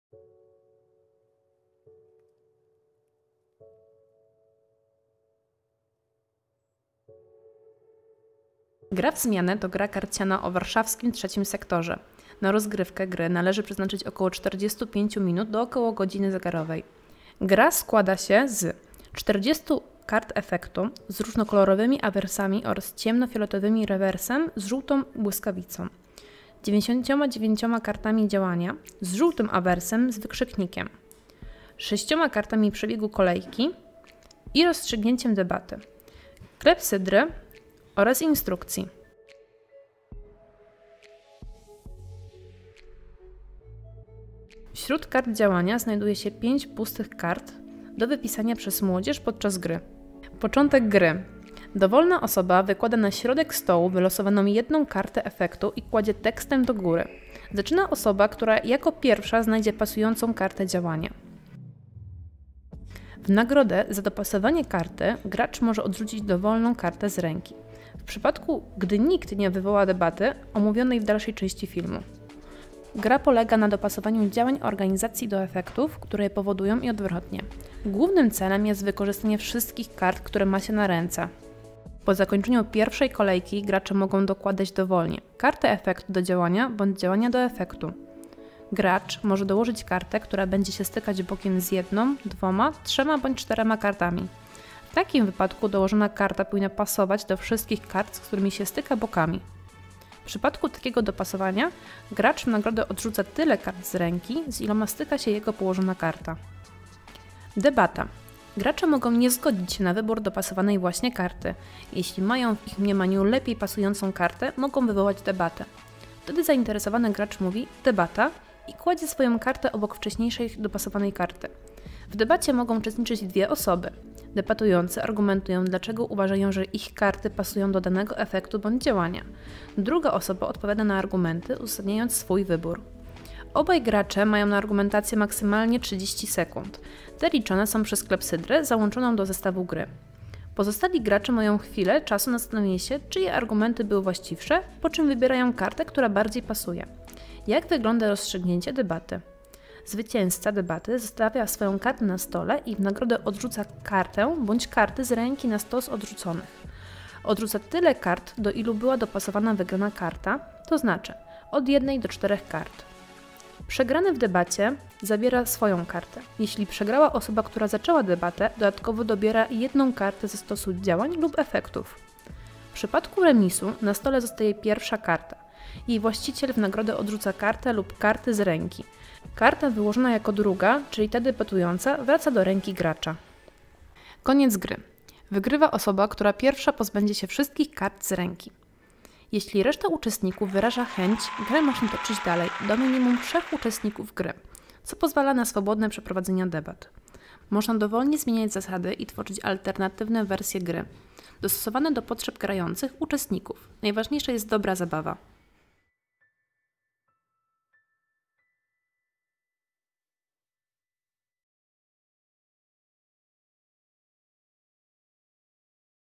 Instrukcja do "Gry w zmianę"- Pozarządownik szkolny
Film instruktażowy do "Gry w zmianę" czyli karcianej gry o organizacjach pozarządowych.
Gra powstała w ramach projektu „Pozarządownik szkolny 2.0” realizowanego przez Fundację Pole Dialogu i Centrum Edukacji Obywatelskiej.

Projekt finansowany przez m.st. Warszawa.